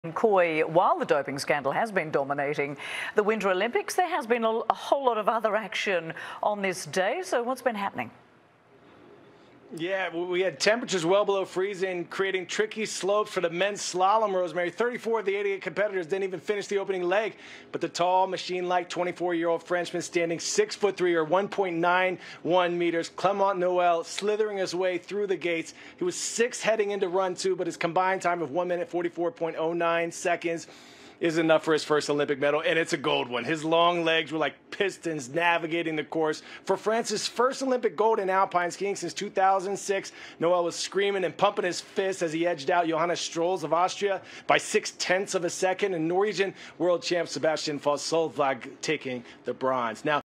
France’s Clement Noel Wins Olympic Gold In Slalom
Live from Atlanta, Rosemary Church brings context and clarity to the day's top stories from around the world.